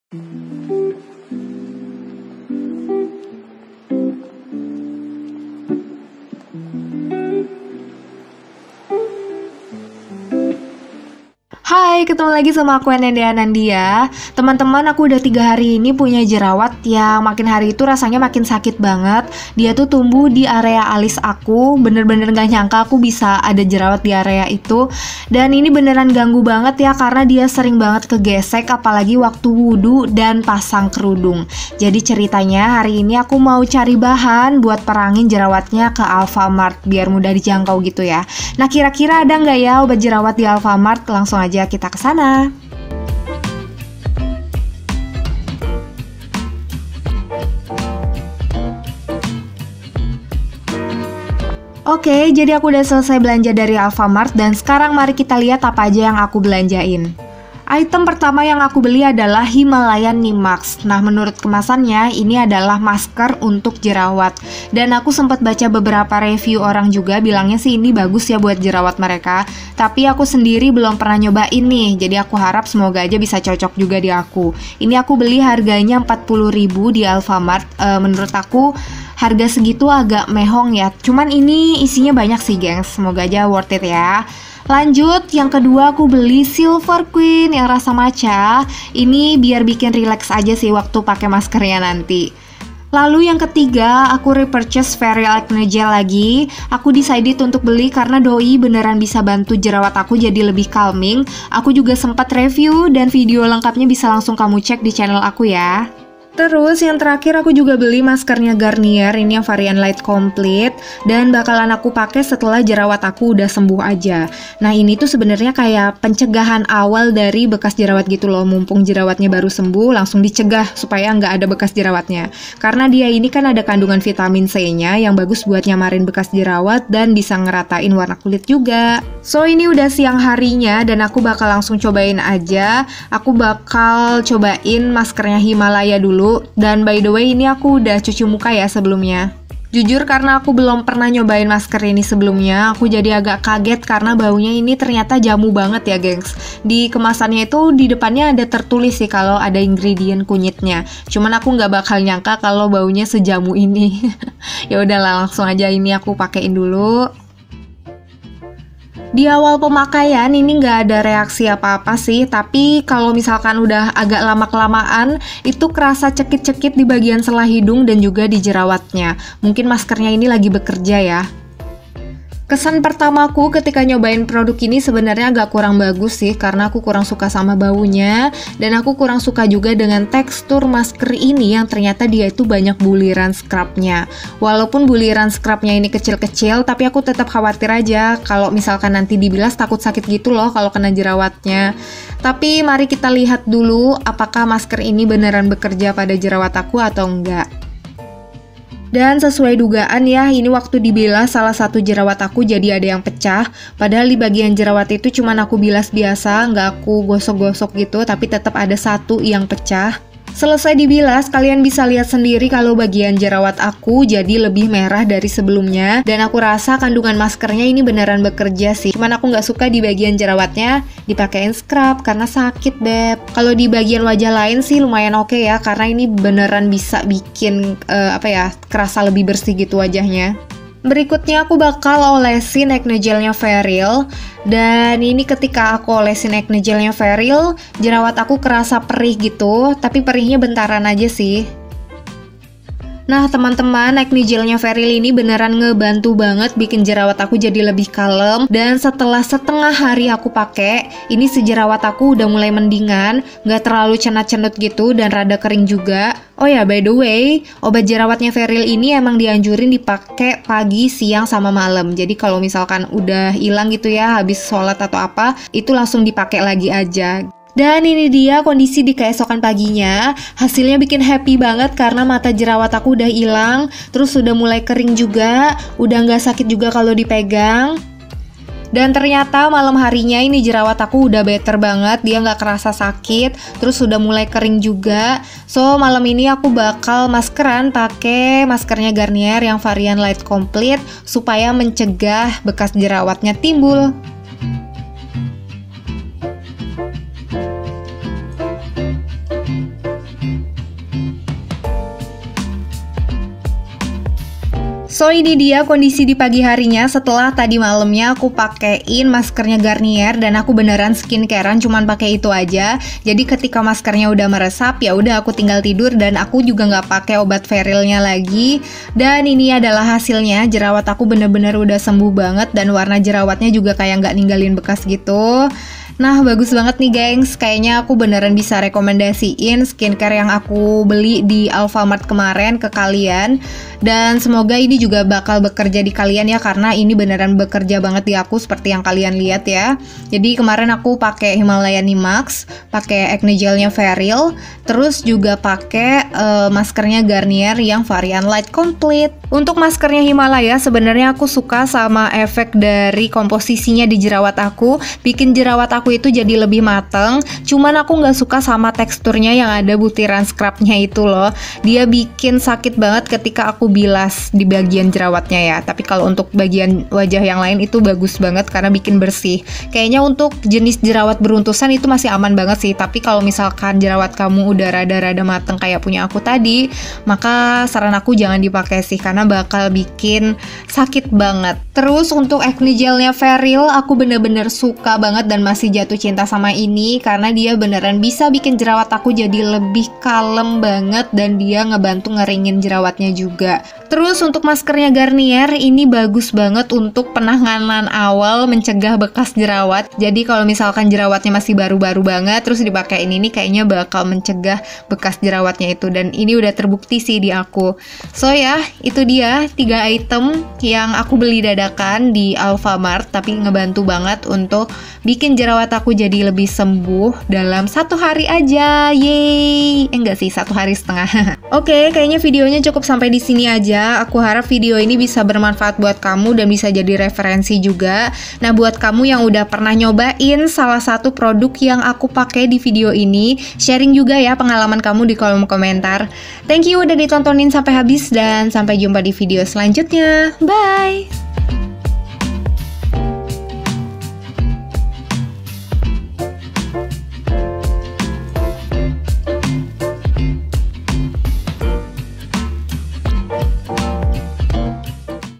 Menghilangkan jerawat + bekas jerawat dalam 1 HARI 😍
Terdadakan banget cari skincare jerawat di Alfamart, untungnya hasilnya cucok di aku.. jerawat ilang dalam 1 hari.. Alhamdulilah 😍 ada yang pernah coba produknya juga? share ya di komentar 😊

buat yang mau beli online aku kasil alternatif tempat belinya ya 😊 tentu aja harganya jauh lebih murah 😭

⚠️ CUMA SAMPAI 12 Des 2020 ⚠️

Mari berteman di ig @dianandyaa